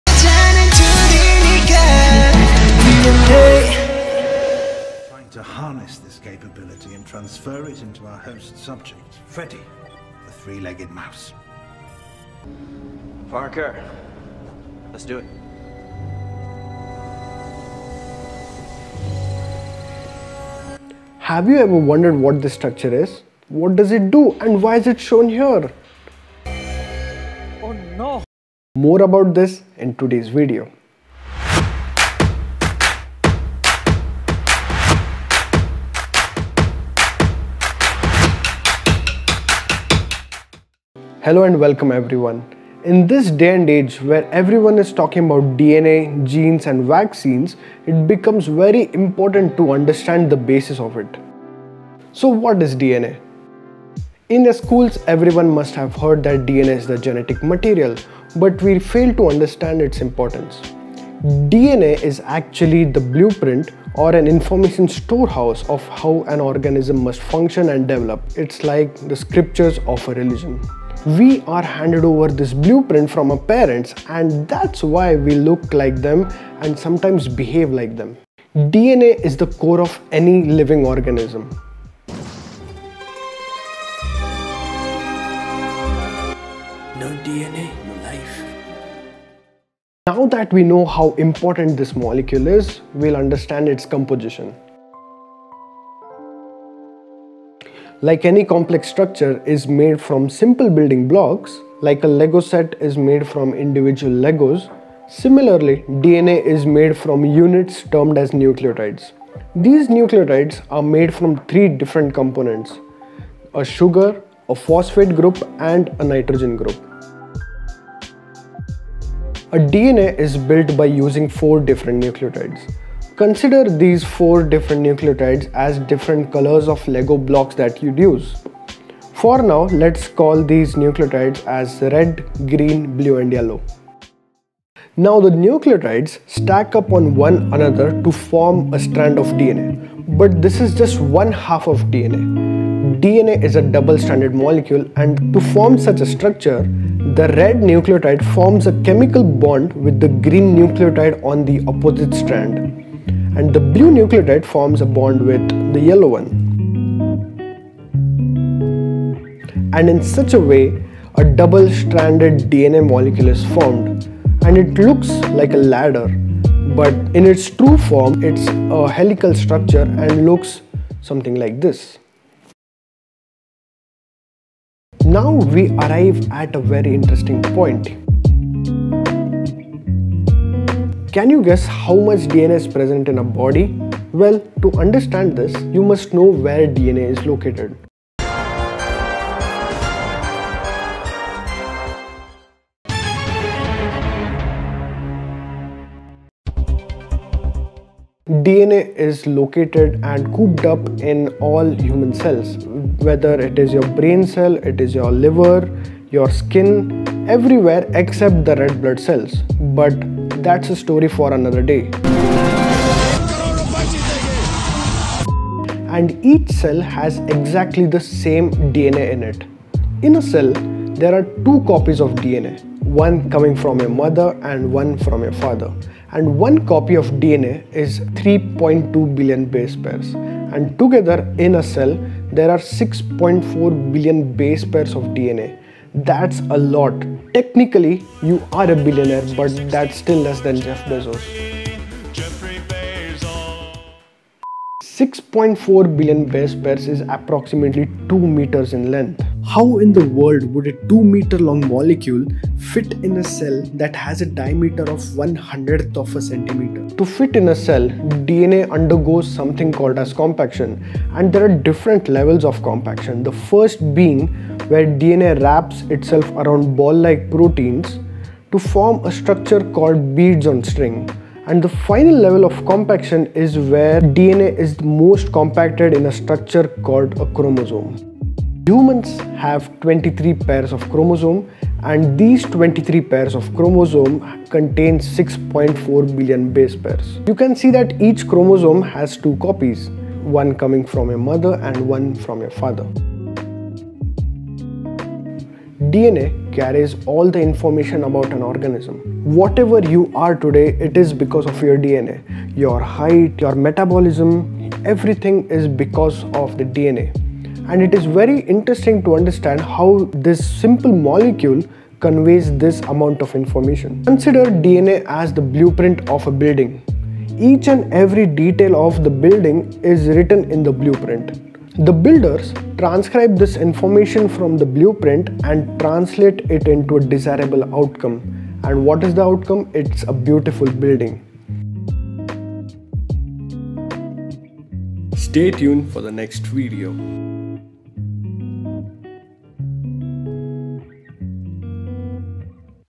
DNA - The Blueprint of Life
In this first video of a video-series, we introduce the most important component of life, the DNA.

Further reading -